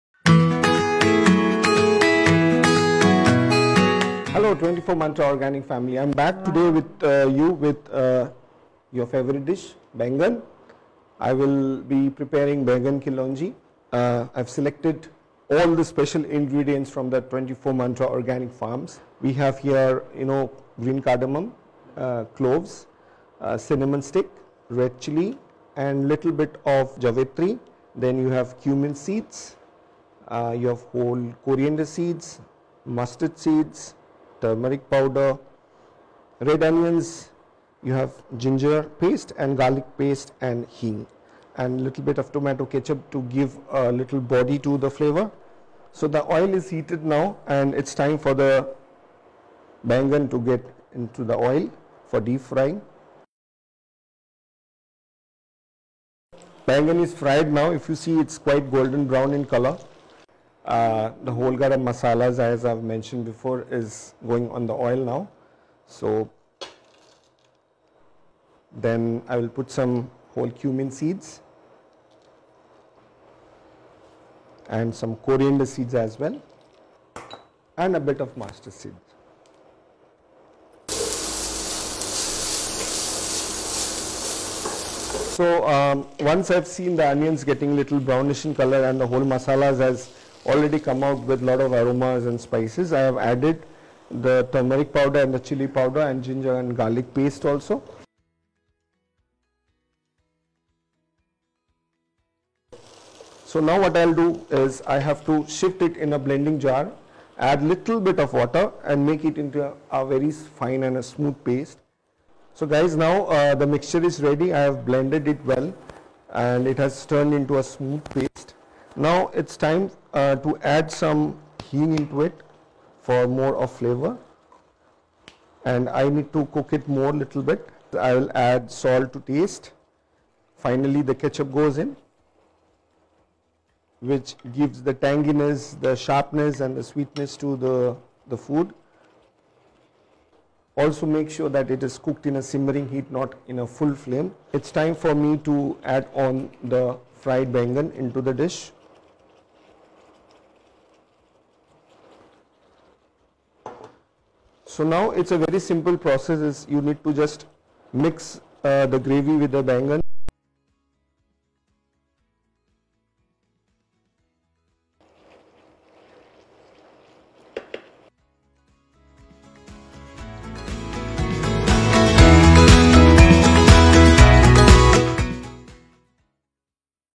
Delicious Baigan Ke Lonje (stuffed baby aubergine/ brinjals curry)
Craving for some delicious Baigan Ke Lonje (stuffed baby aubergine/ brinjals curry)?

Make it all the more appetising and enhance the overall flavour with the authentic, aromatic spices of 24MantraOrganic.
Here's how you can make this lip-smacking recipe:
-Slit the brinjals vertically and pour in a pan of oil for deep frying.
-Fry the brinjals till it attains a golden brown color. Set it aside.
-Add whole garam masala to the oil, followed by cumin, coriander and mustard seeds.
-Further add the onions and start sautéing.
-When the onion turns brownish and the masala mixture emits a delicious aroma, add the turmeric powder, chilli powder along with ginger and garlic paste.
-After frying the mixture for some more time, shift it in a blending jar.
-Add a little bit of water and blend it into a very fine, smooth paste.
-Then add the smooth paste back into the pan and add some asafoetida, salt to taste and also a little bit of tomato ketchup to enhance the volume of the dish.
-On a low flame, cook the paste while stirring it continuously.
-Finally add the fried brinjals that were kept aside and gently mix the gravy.
Voila! Your baigan ke lonje is ready to be served hot! 💗😍😋